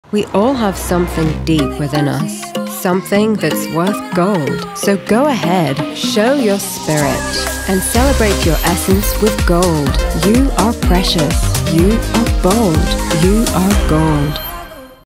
You Are Gold I Traveller I 15 sec
Celebrate your spirit with gold

You are a wanderer. Someone who always treads the roads less taken and finds herself along the way. And every time you stop to take a breath, you can hear the next adventure, calling out your name.

So, let your spirit wander free, and know that you are precious. You are bold. You are gold.